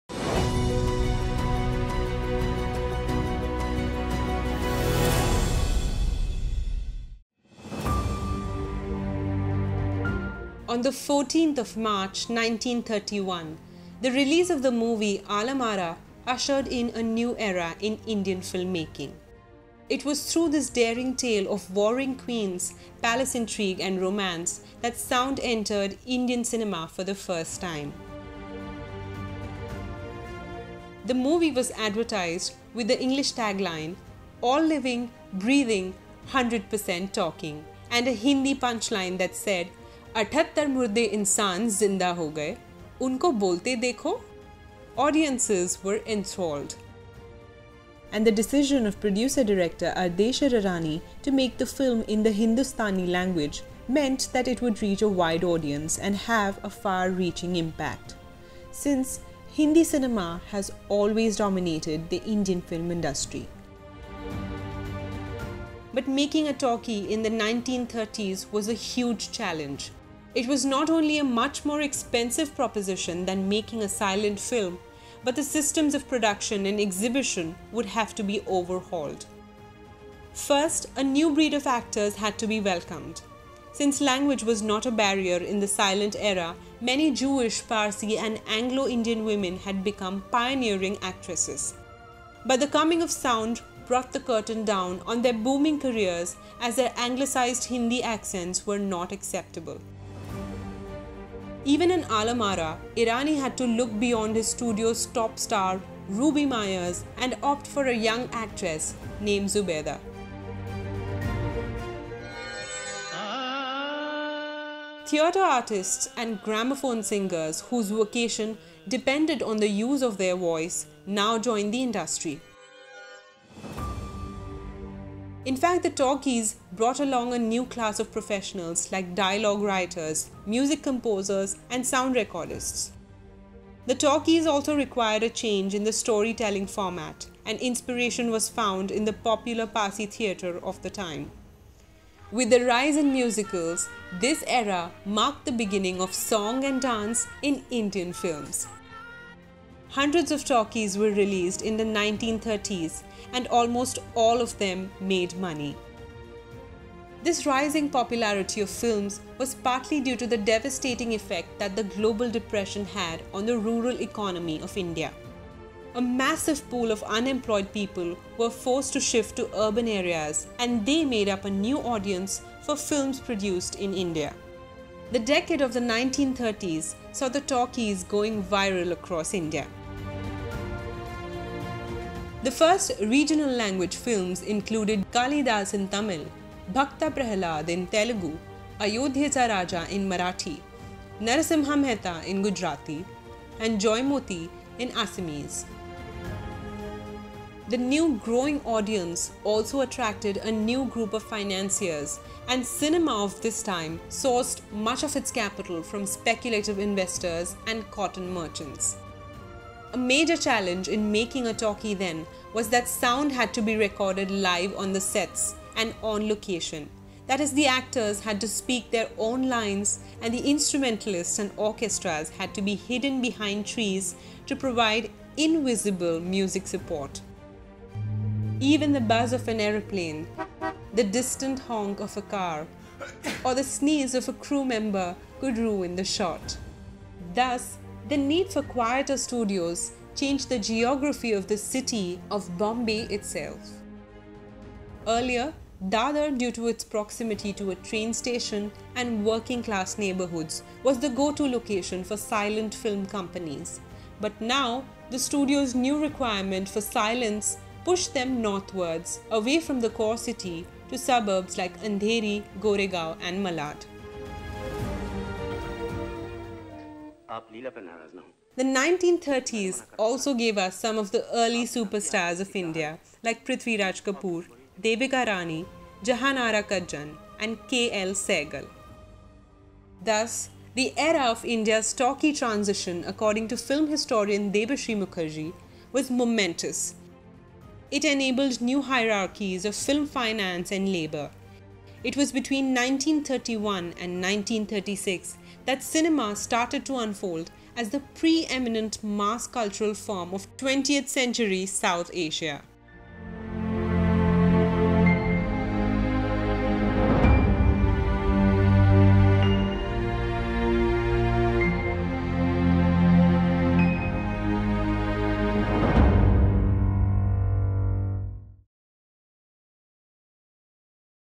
The Coming Of The Talkies | Milestone | Making of Modern India
Ninety years ago, the first ‘All-Living, Breathing, 100 per cent talking’ movie, Alam Ara, released in India. But adding sound to films didn’t just bring them alive, it changed every aspect of movie making. Catch the story of India’s celluloid sound revolution in this edition of Milestone...

to rediscover stories that are deeply rooted in our history and culture.